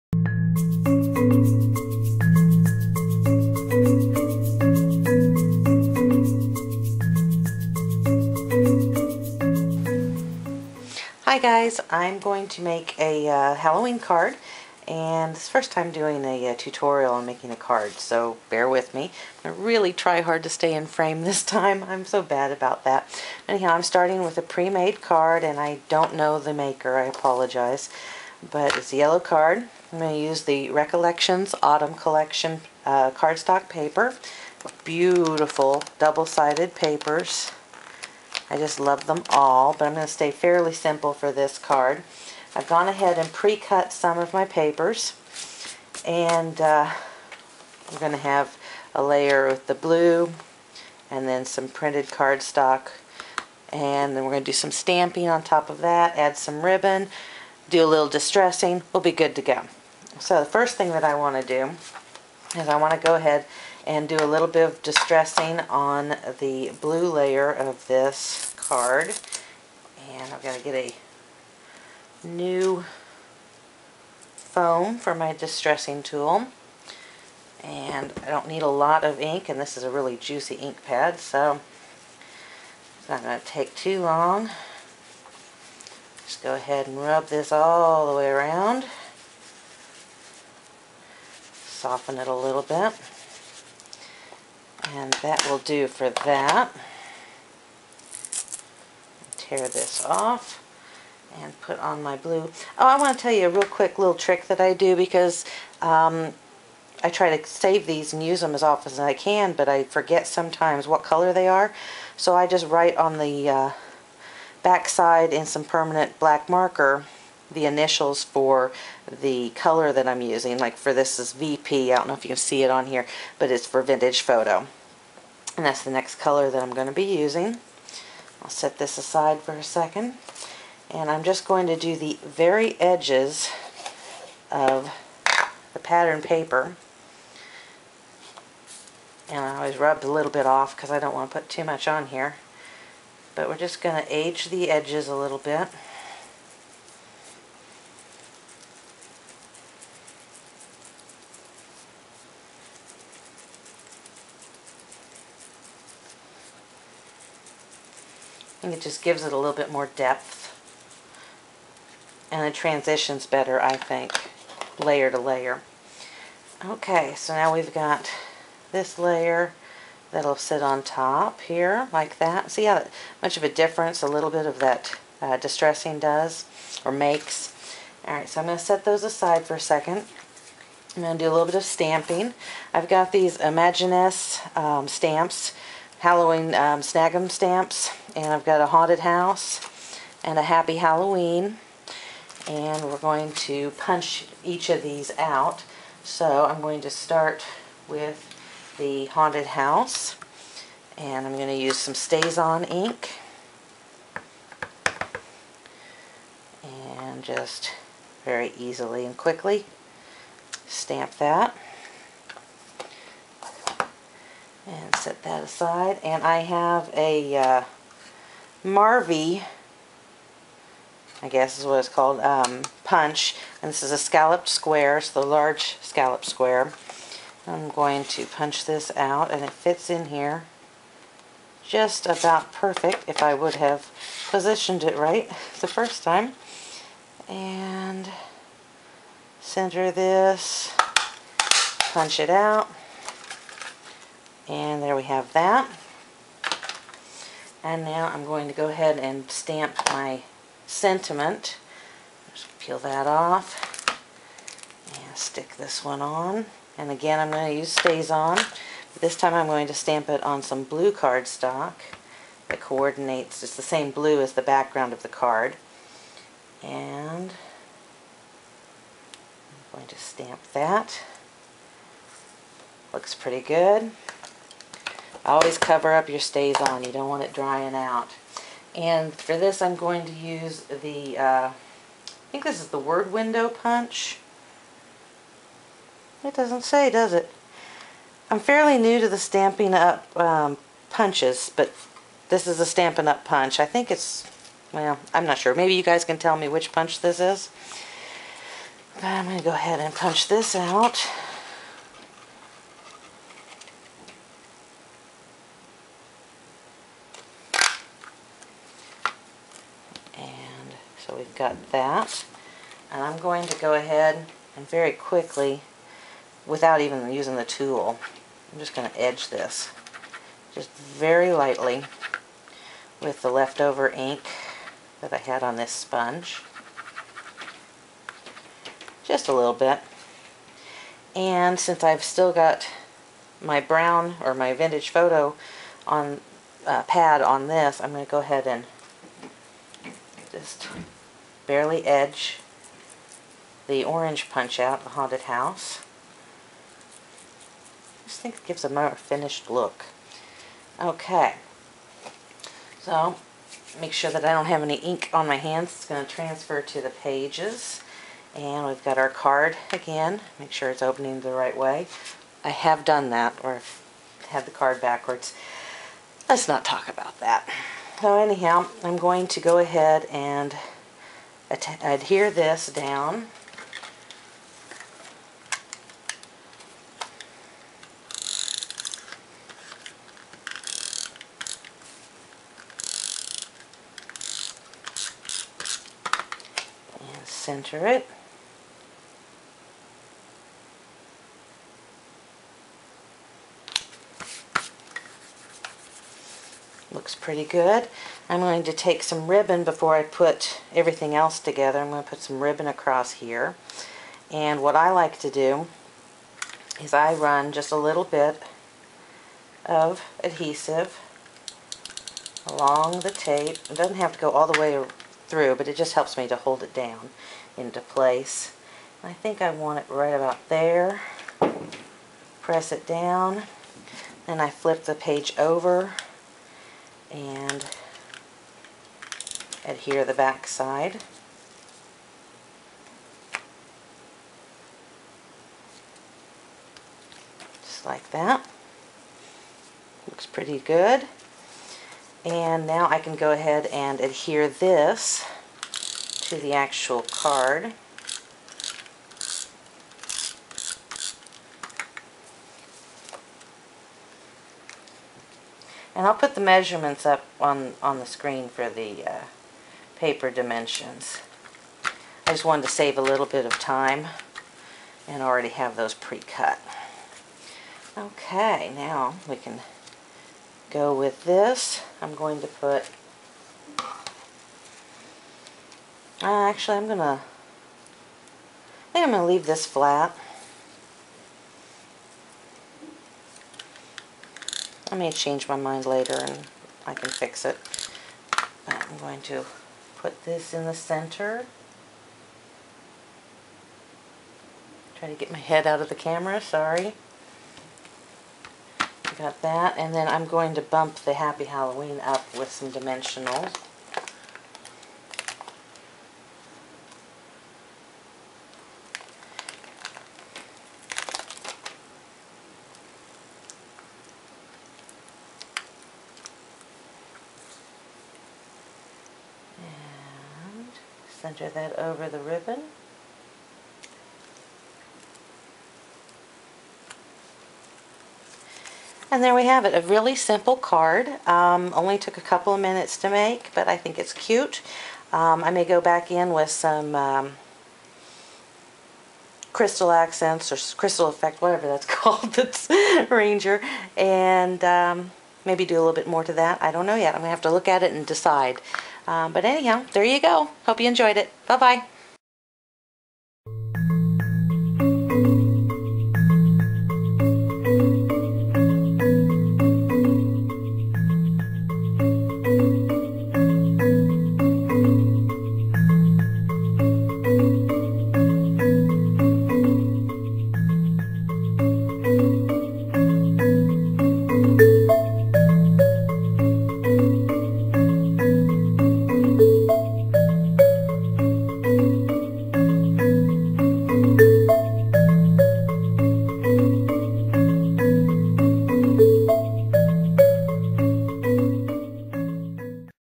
Halloween Card 2009
Just a quick holiday card that I made this evening.  Enjoy!